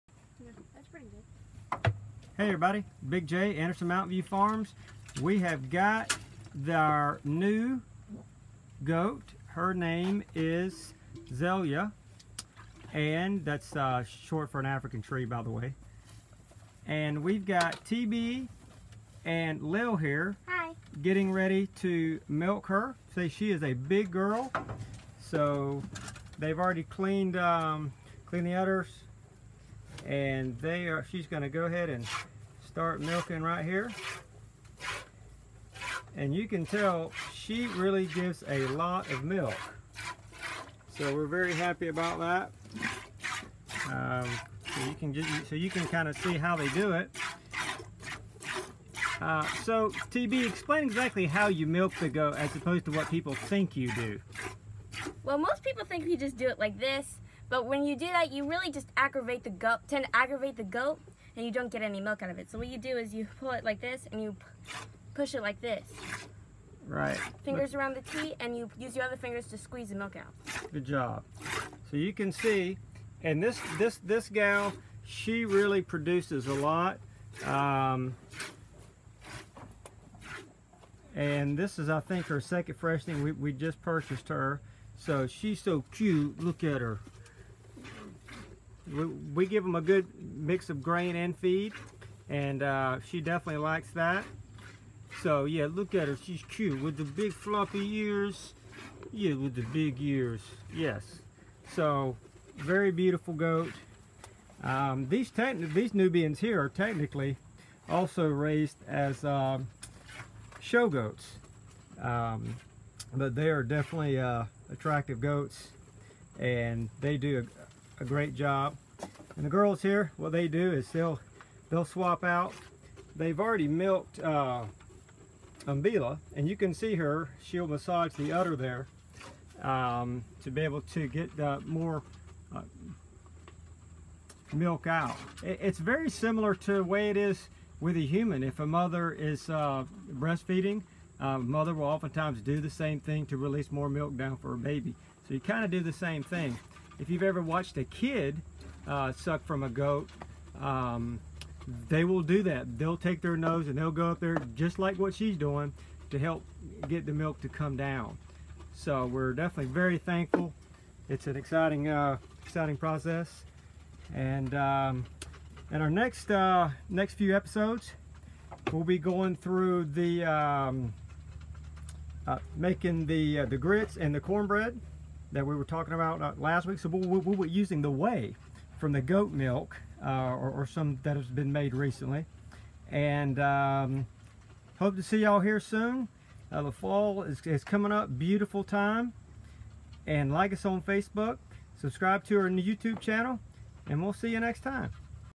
Anderson Mountain View Farms: Milking Our New Beautiful Nubian Goat Zelia!--Episode 59
Hello everyone, in this episode, TB and Lil milk our new beautiful nubian goat Zelia, short for Afzelia a brown African tree. She is actually from a good registered show goat breeding stock like Umbila, our other goat named after a different brown African Tree. The girls do a great job milking her, and she really produces a lot of good quality milk. Hope you can come by and see us soon, especially as the Fall approaches!